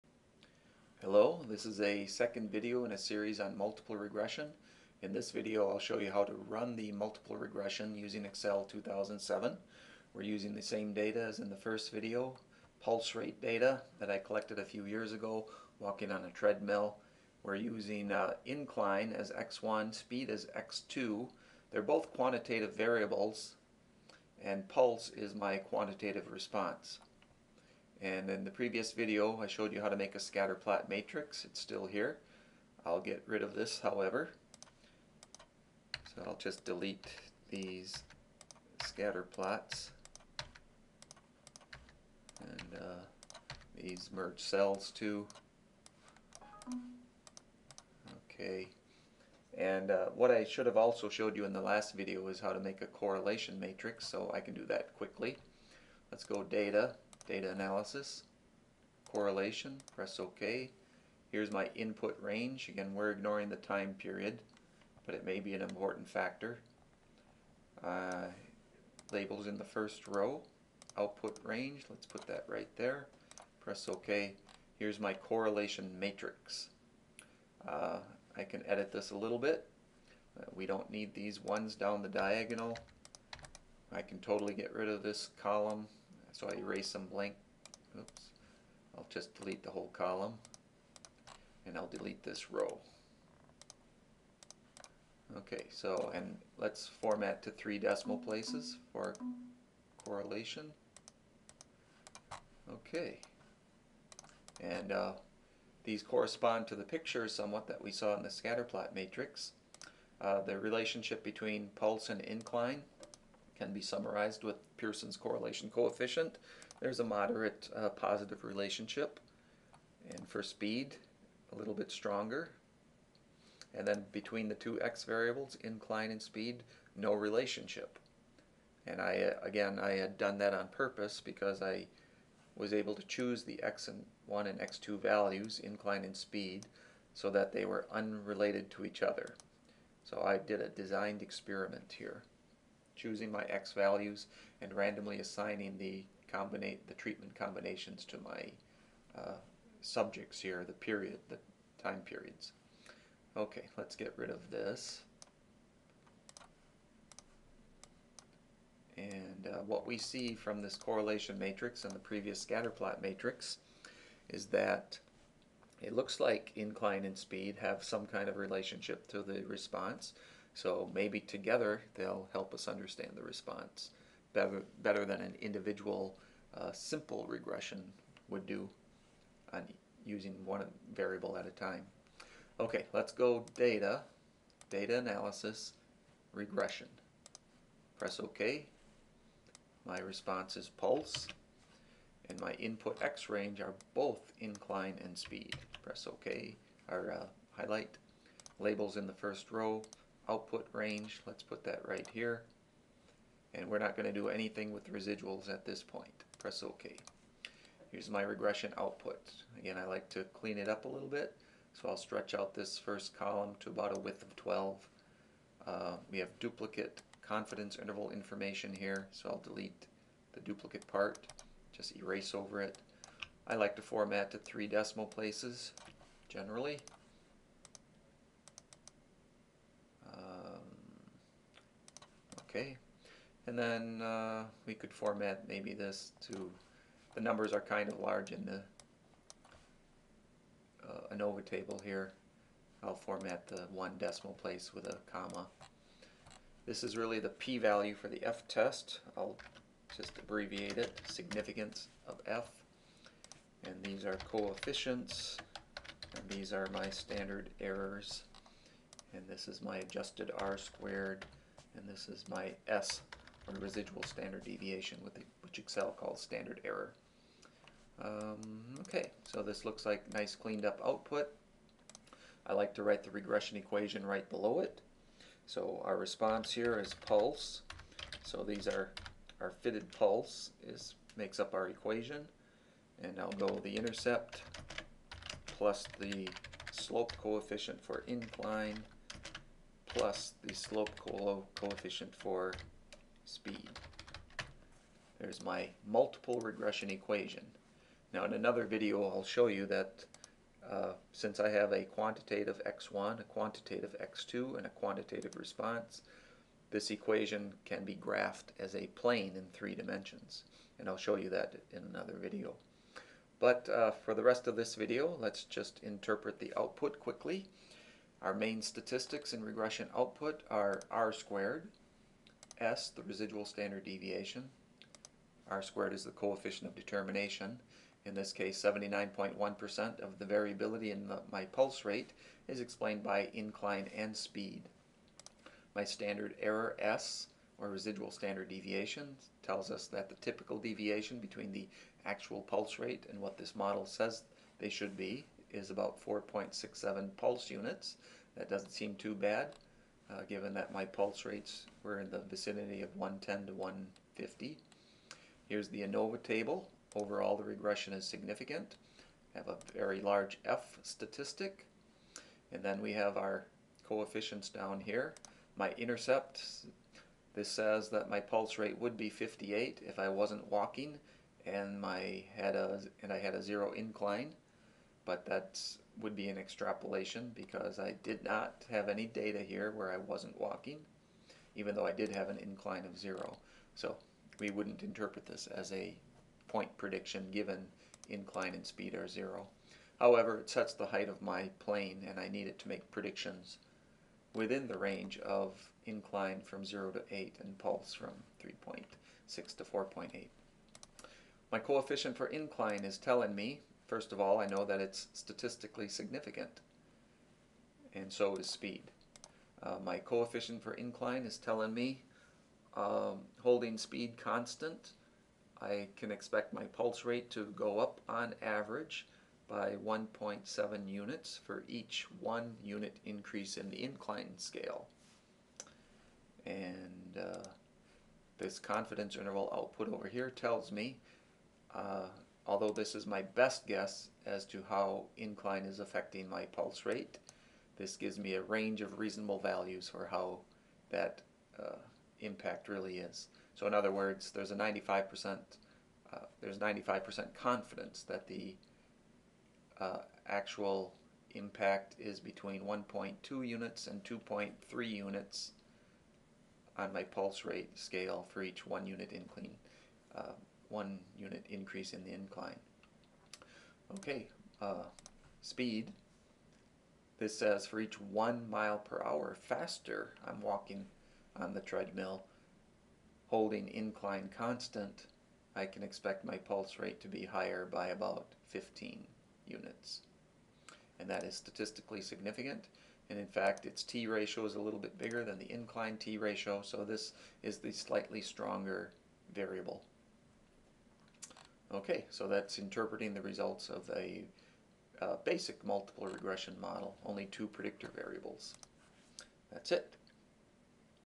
How to Run a Multiple Regression in Excel 2007
I take some pulse rate data and relate it to incline and walking speed on a treadmill via a multiple regression analysis.  In addition to showing you how to get the multiple regression output, I interpret the results.